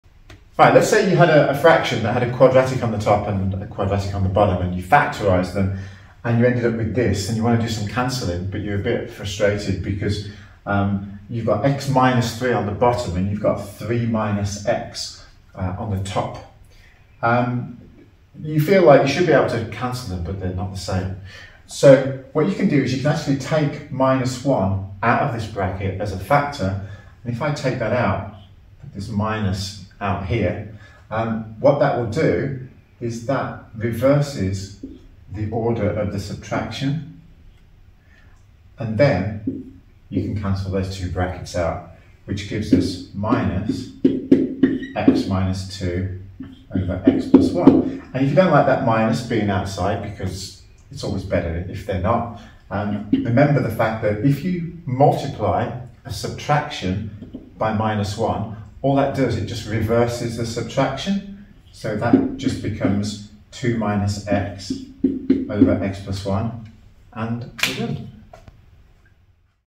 Algebraic fractions 2
Cancelling when two brackets are almost the same but not quite!